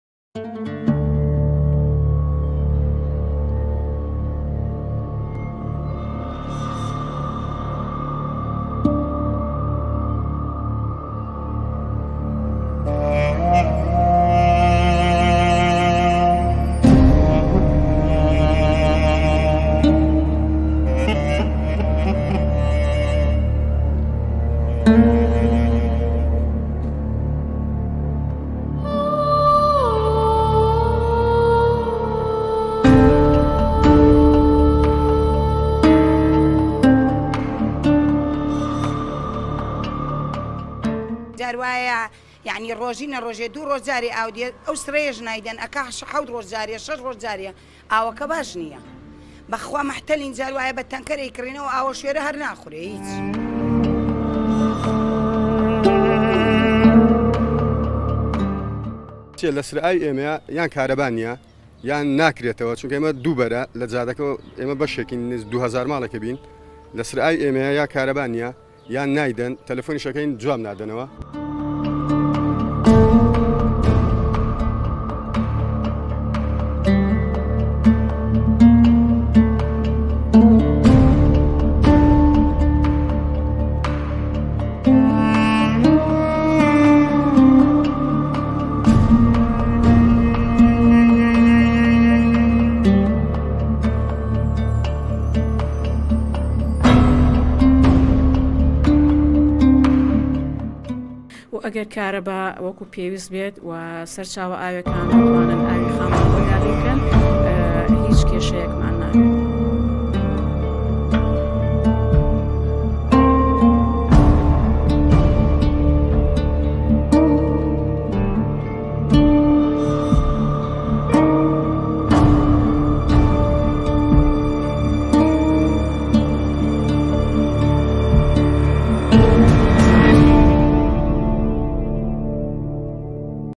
Rudaw: Thousands of families left without clean water in Kirkuk
Water shortages are making life difficult for residents of Kirkuk. More than 2,000 families in the city's Kurdistan neighbourhood are among those affected. "The water comes only once every two days. This sometimes reduces to once every six to seven days. The water isn't clean, though. When we urgently need water, we buy water tanks, but the water is too salty to drink," local Kharman Mahmood told Rudaw.  Water shortages are a problem across Iraq and the Kurdistan Region, and have been exacerbated this year by drought. Kurdistan Region officials have warned that the region is experiencing a "water crisis", while a lack of water has wreaked havoc on farming in sourthern Iraq. Authorities in Kirkuk said a lack of electricity is the problem. "If there is sufficient electricity and the water resources provide us with water, we would have no problems," said Jihan Ibrahim, deputy director of Kirkuk's water directorate. Kirkuk consumes more than 250,000 cubic meters of water every day, according to Ibrahim.  Source